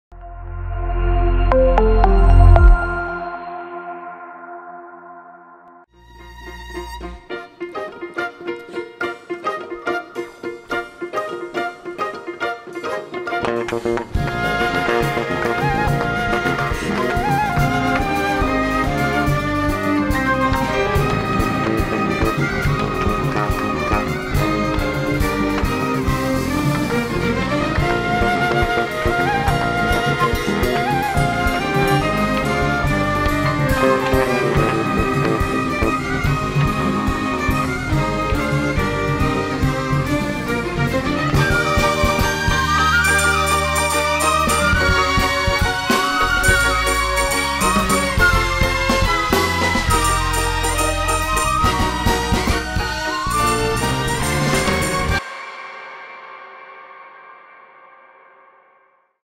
Art-East Media Group üçün hazırladığımız "Musiqli Proektor" adlı konsertin anonsu
Rejissor : Vüqar Nəsirov
Səs köməkçi : Kamal Hüseynov
Montaj : Nurlan Qasımov

BU VİDEO "RAMANA MEDİA CENTER-ə MƏXSUSDUR !